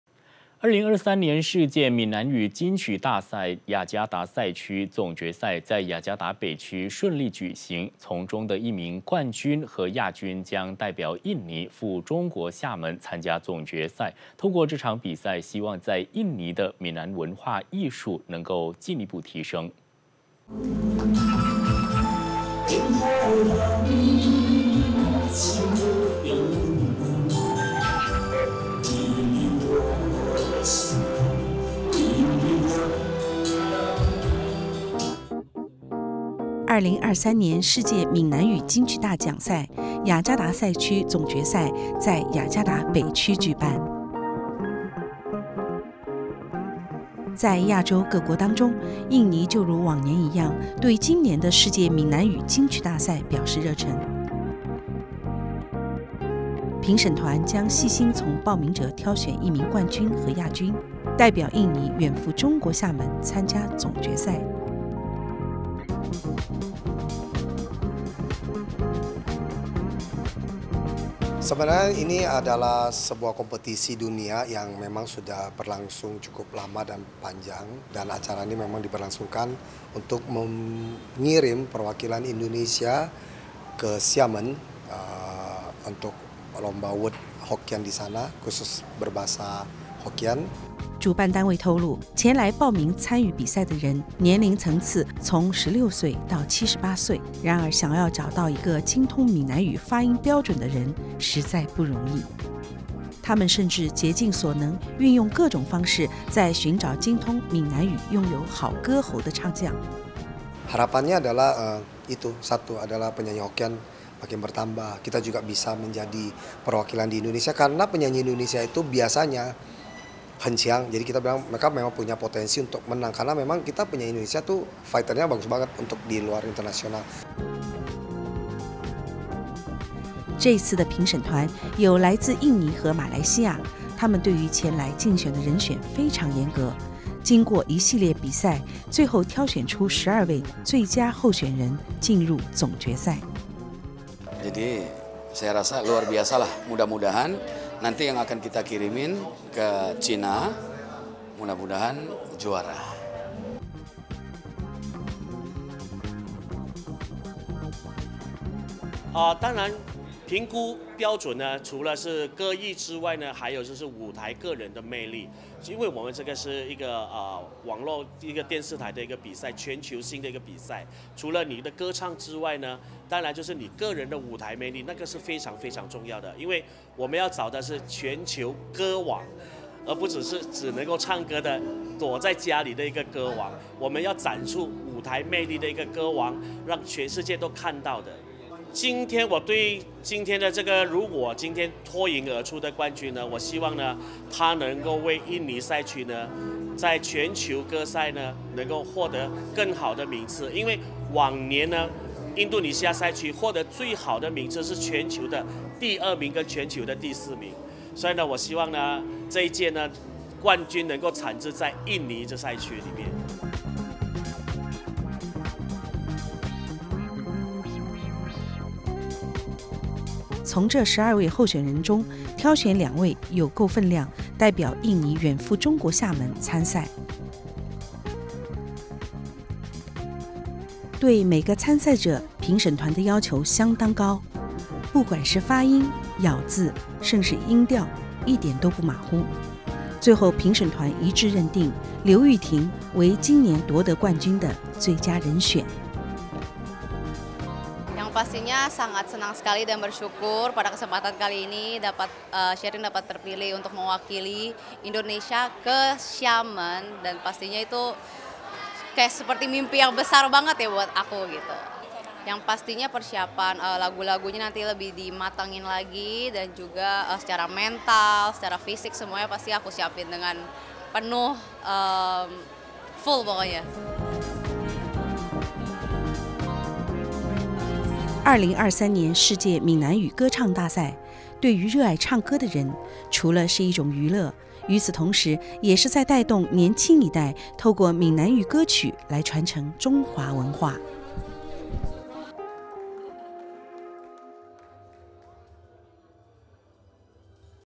Memperkenalkan Bahasa Hokkien Lewat Musik | DAAI Mandarin 大愛與您分享
Final Kompetisi Menyanyi “World Hokkien Golden Melody Awards” diadakan di Jakarta Utara. Pemenang juara pertama dan kedua akan
mewakili Indoneasia untuk mengikuti tahap final di Xiamen, Tiongkok. Diharapkan melalu kompetisi ini dapat mengembangkan seni budaya Hokkien di Indonesia.

2023年世界閩南語金曲大獎，雅加達賽區總決賽，在雅加達北區舉辦。從中的一名冠軍和亞軍，將代表印尼，赴中國廈門參加總決賽。透過這場比賽，希望在印尼的閩南文化藝術，能進一步提升。